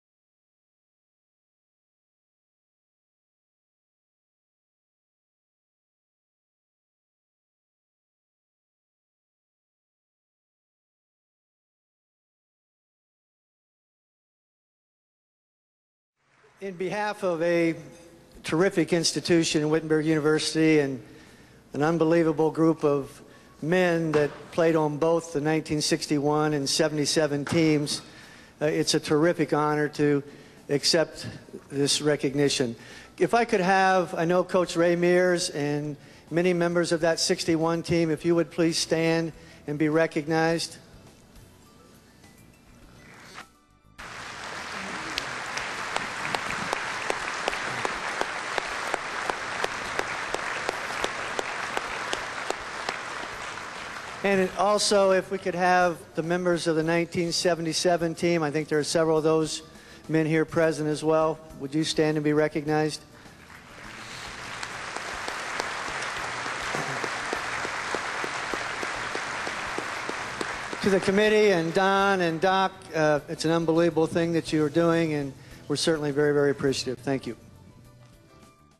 Wittenberg University, 1961 & 1977
Wittenberg University's 1961 and 1977 men's basketball teams were enshrined in the Ohio Basketball Hall of Fame's Charter Class of 2006.